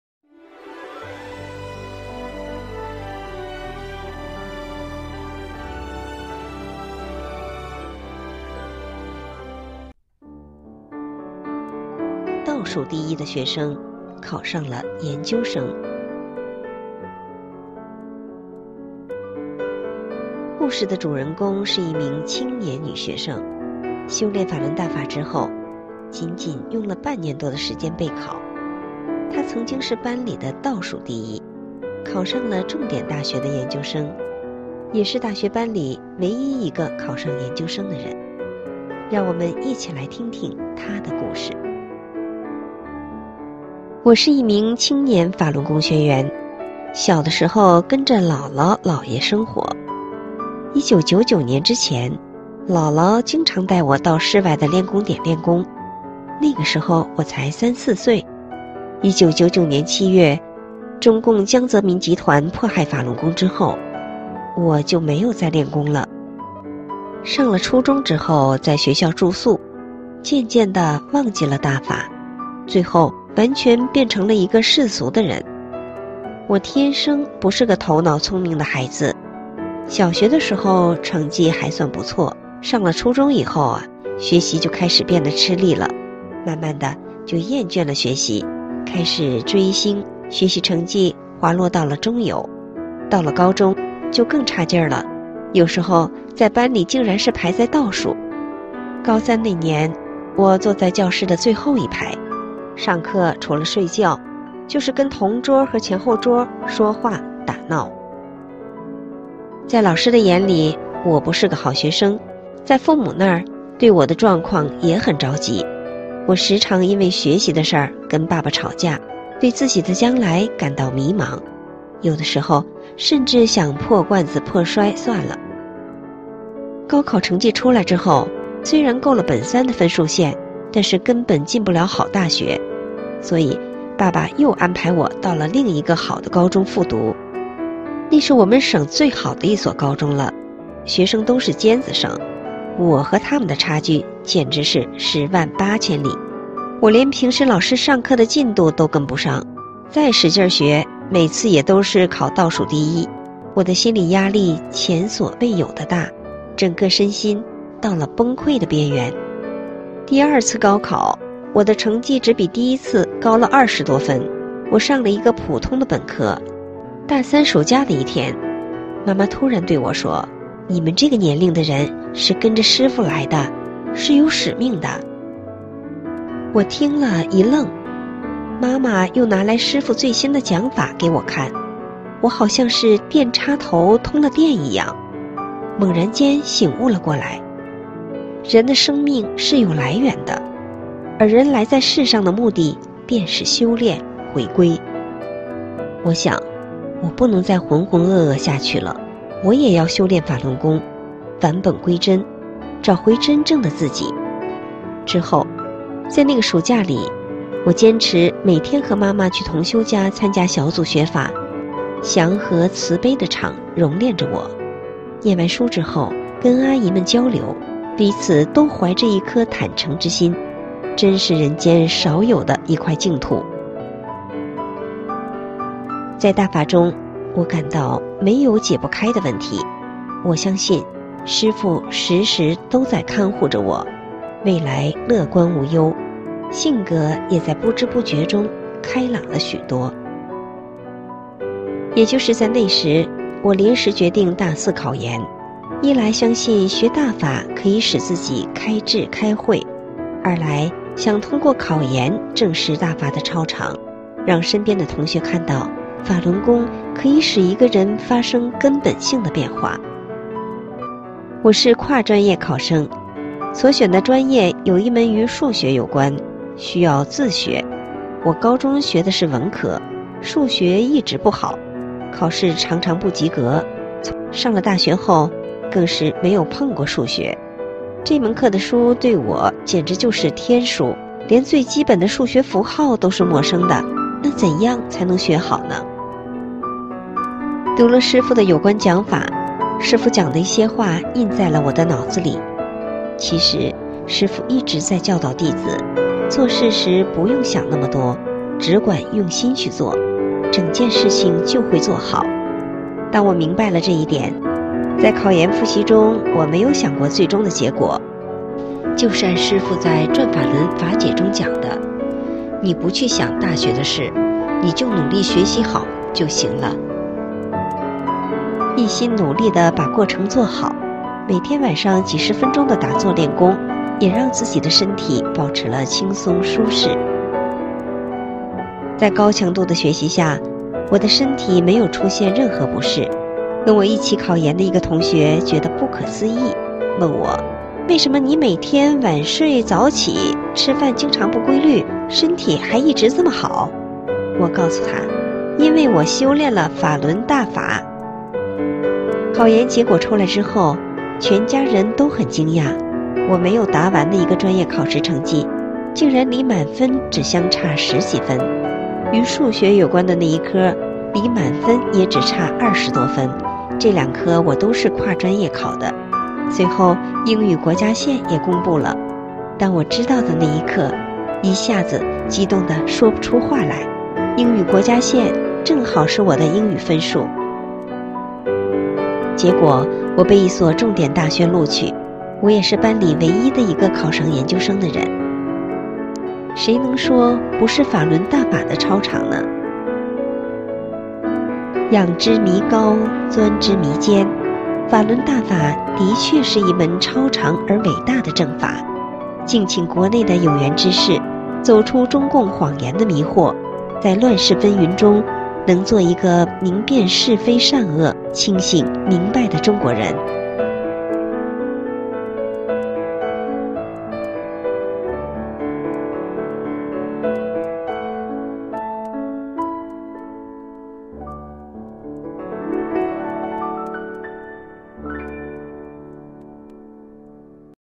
倒数第一的学生考上了研究生 | 这是什么原因呢 ? 快来看这神奇的故事 | 法轮功开智慧
👍 -不要忘了点赞👍 ！

我是一名青年法轮功学员，小的时候跟着姥姥、姥爷生活，由于他们都修炼法轮功，我从小就得了法，知道法轮功教人真、善、忍，做好人。

一九九九年之前，法轮功在中国可以自由传播，姥姥经常带我到室外的炼功点炼功，那时我才三、四岁。一九九九年七月中共江泽民集团迫害法轮功后，我就没再炼功了，只是依稀记得那时电视里天天诬蔑大法，我每次听到都很生气，就像触碰了自己的底线一样。

上了初中以后，我在学校住宿，渐渐的忘记了法，最后完全变成了一个世俗的常人。直到二零一六年大三那年，妈妈的一番话和师父的经文点醒了我，使我又从新回到了大法中来。从决定修炼法轮功的那一天起，我的人生开始了一个新的阶段，改变是真实不虚的，其中最不可思议、也最值得讲述的是：我这个在班里曾经成绩倒数第一的学生，竟一跃考上了重点大学的研究生。

以下就是我的故事。